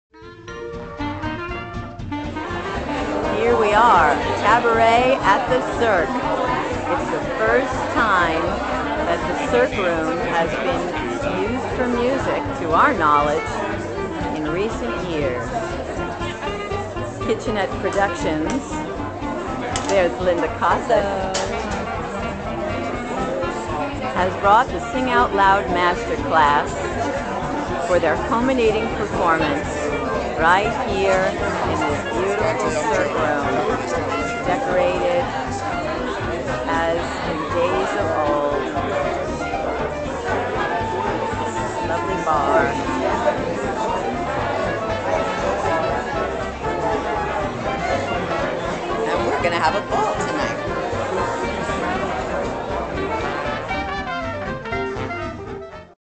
Cabaret at the Cirque Room_Fairmont_Sing Out Loud
SING OUT LOUD! A performance workshop for singers with master teachers Lua Hadar, Linda Kosut & Barry Lloyd,
presents, Cabaret at the Cirque. Sing Out Loud! is an 8-week master class culminating in a final concert, produced by Kitchenette Productions, San Francisco, CA.

In our programs, students have the opportunity to work on songs in a master class setting, address interpretation of lyrics, stage presence, mic technique, verbal introduction to a song, how to work with instrumentalists, how to sell the song in performance.

Cabaret at the Cirque  bought music back to the historic Cirque Room at San Franciscos renowned Fairmont Hotel on November 21, 2009.